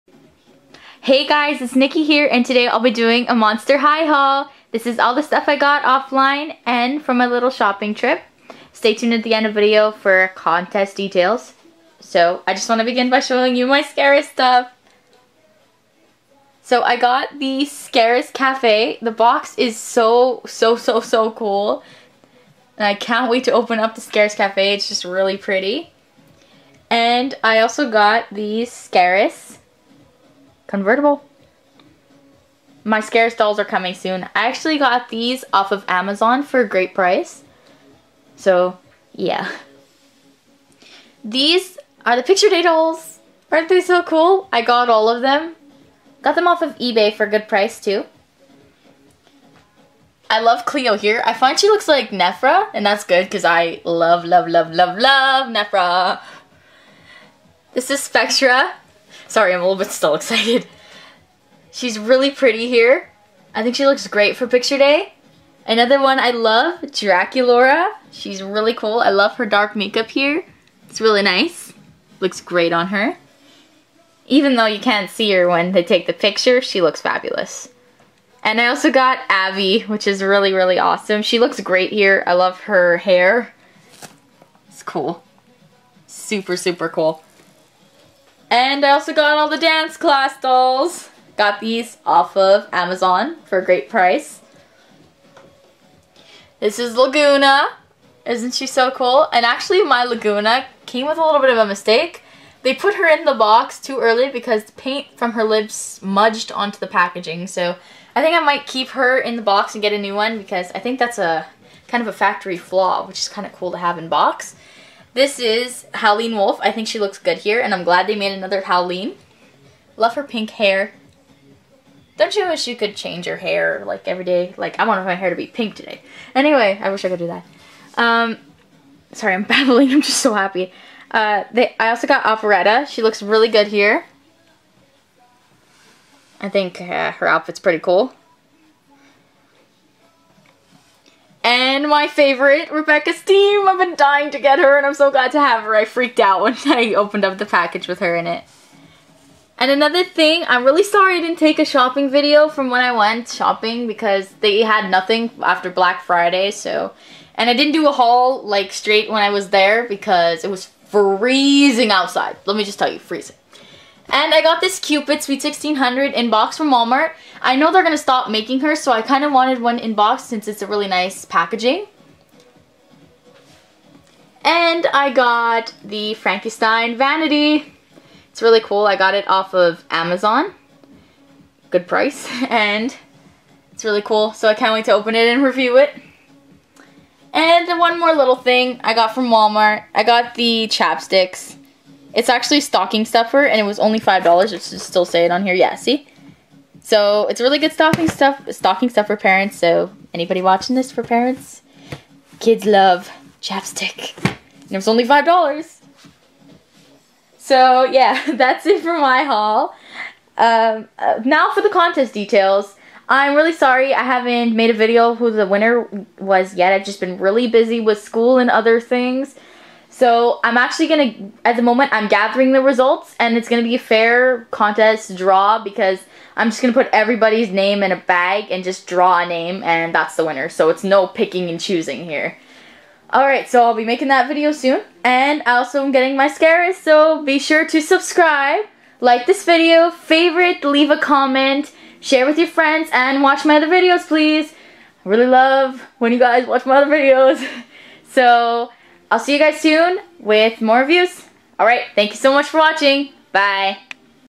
Monster High Haul #5 Scaris Picture Day Dance Class & Cupid!!!!!!! :D
Dance Class Howleen Wolf Robecca Steam Operetta Lagoona Blue!!
Picture Day Spectra Vondergeist Cleo de Nile Draculaura Abbey Bominable!!
C.A. Cupid!!
Frankie Stein Vanity!!
Scaris  convertible & Cafe Cart!!